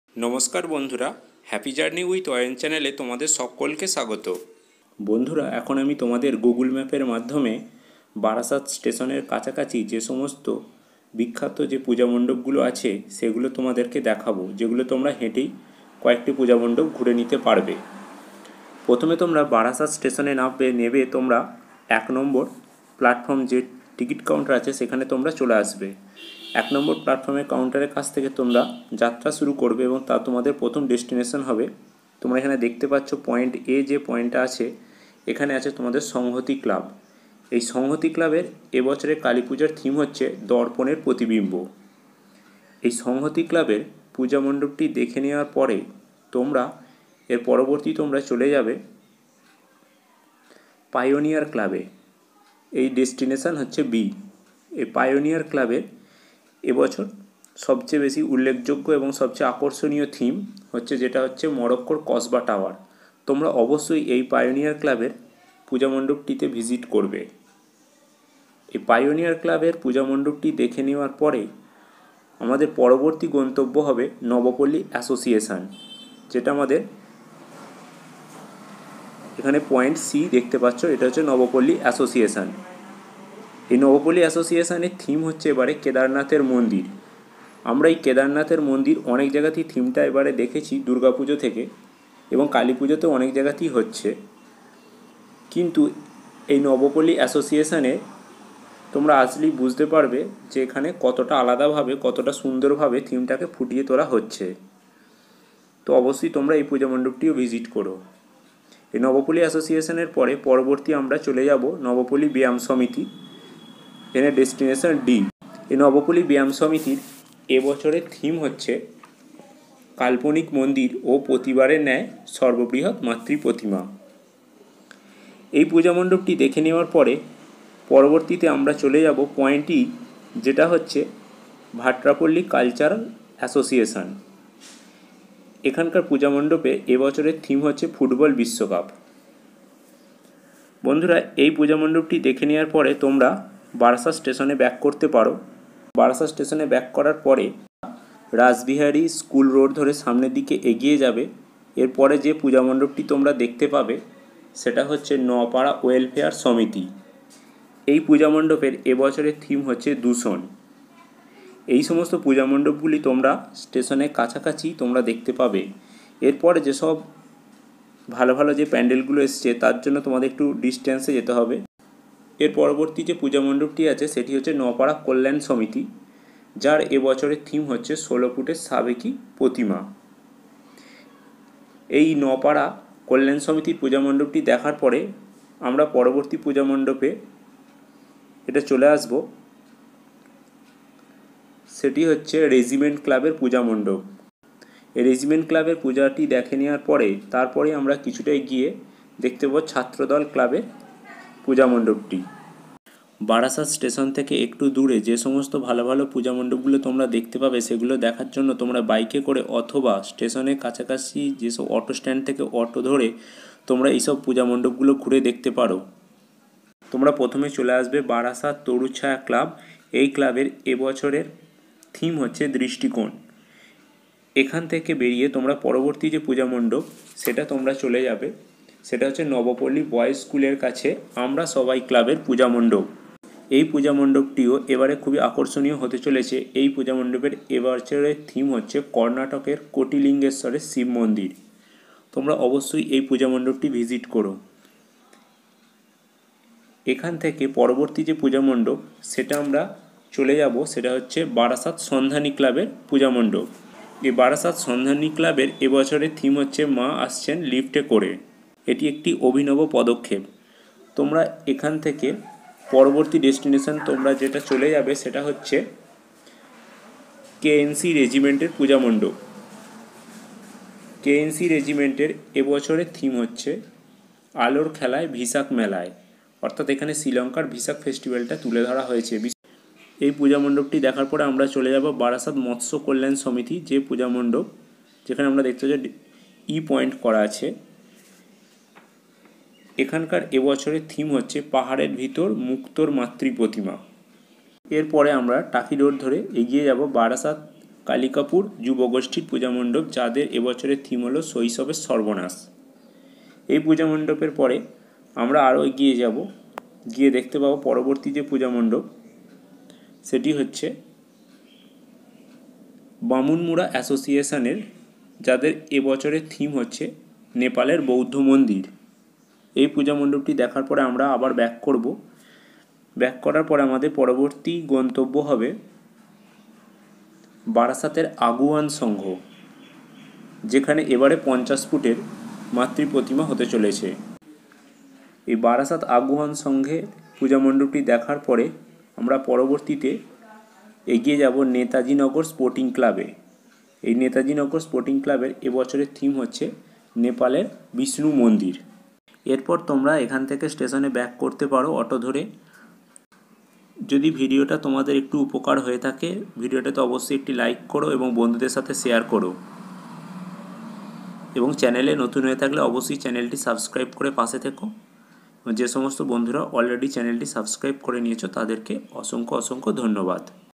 Barasat Kali Puja Route Map/বারাসাত কালী পূজার পথ নির্দেশিকা ২০২২ with Google map.
বারাসাত কালী পূজার পথনির্দেশিকা ২০২২ with Google map.
Barasat Kali puja route map 2022.

নমস্কার বন্ধুরা এই ভিডিওতে আমি দেখিয়েছি, কম সময়ের মধ্যে কিভাবে বারাসাত রেল স্টেশন থেকে এবছরের আকর্ষণীয় এবং বড় বড় কালী পূজা মন্ডপ গুলো পরিদর্শন করবে সাথে Google location দেওয়া আছে।